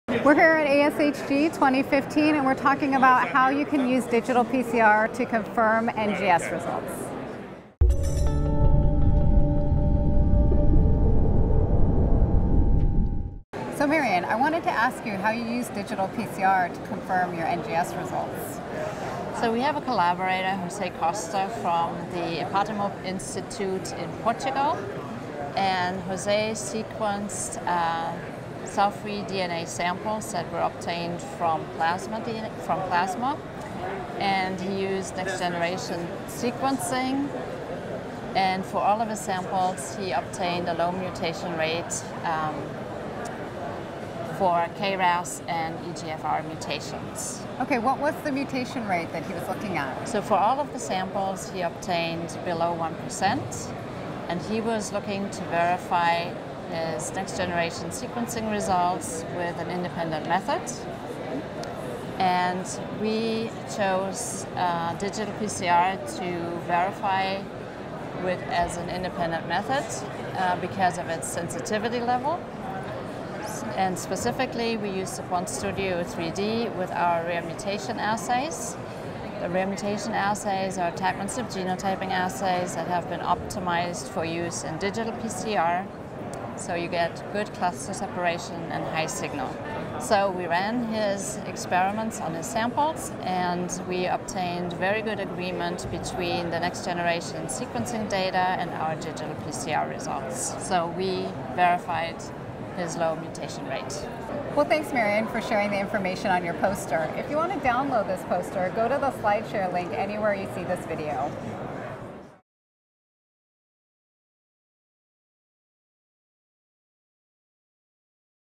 Using Digital PCR to Verify NGS for Cancer Research | ASHG 2015
Marion Laig of Thermo Fisher Scientific answers questions about her poster that was presented the 2015 American Society of Human Genetics annual meeting.

Abstract
We identified mutations in eleven cell free (cf) DNA samples by next generation sequencing (NGS) using the Ion AmpliSeq™ Colon & Lung Cancer Research Panel and the Ion PGM™ System. Since detection of low frequency mutant alleles may not always be called confidently in NGS, we verified results by rare mutation analysis using digital PCR on the QuantStudio™ 3D Digital PCR System as an independent method. We show that frequencies detected are consistent for both methods for low frequency mutant alleles at and below 1%.

Next generation sequencing (NGS) is becoming an increasingly important analysis tool for the discovery of biomarkers in cancer research and to study tumor heterogeneity in samples. Although NGS is an excellent discovery tool, detection of low frequency mutant alleles may not always be called confidently due to an inherent low error rate of the DNA polymerase used (fidelity). It is therefore beneficial to verify detection of rare alleles with an independent
method. In this study, we set out to confirm NGS sensitivity and accuracy detecting low frequency mutations using rare mutation analysis by digital PCR as an independent technology. We present data showing consistency in low level mutation detection in cf DNA samples between two methods: NGS and Rare Mutation Analysis using digital PCR